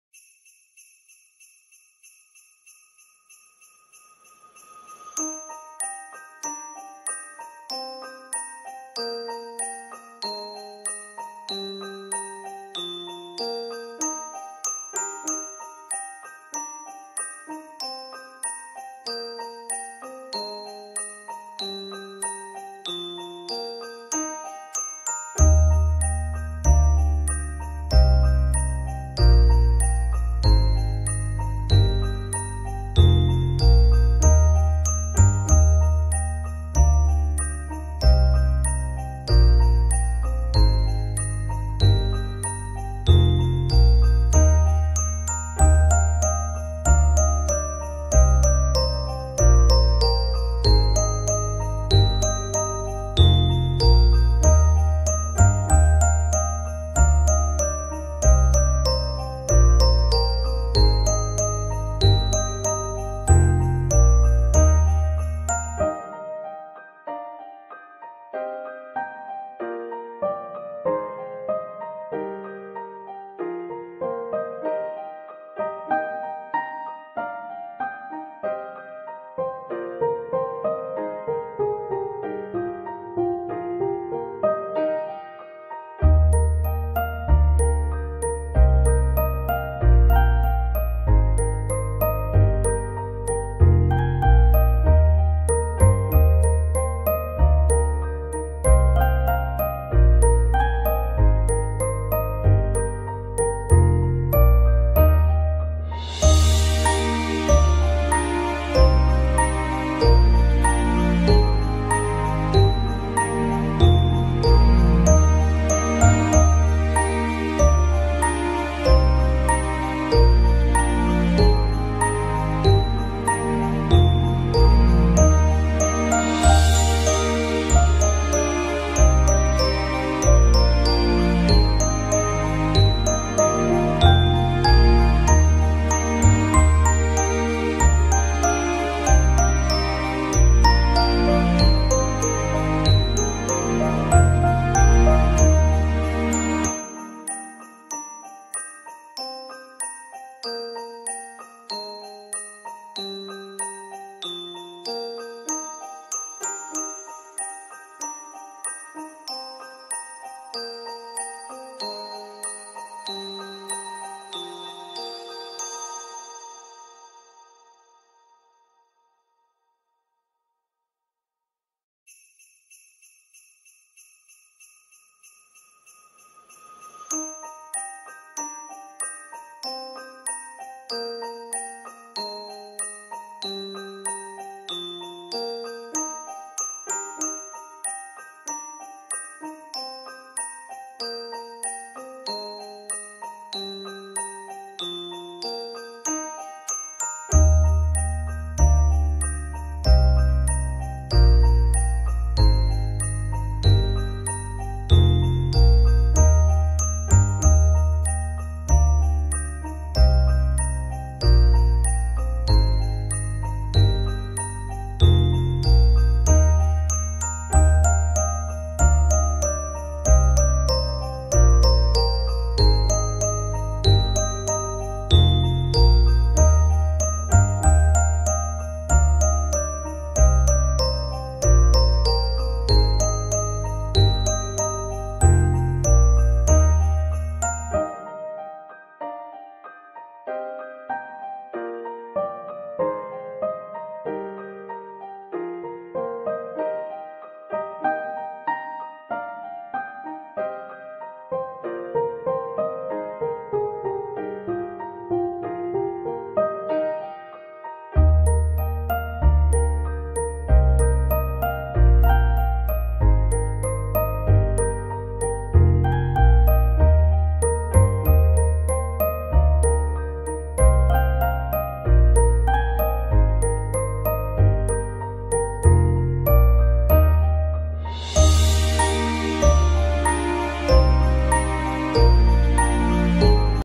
WalterMart Holiday Mall Hours 2022
December calls for your Holiday shopping and get togethers! Here's our Holiday Mall Hours to serve you this Christmas season.

Check this post to know more. 🎄

Be part of WalterMartCommunity to get exclusive updates and deals!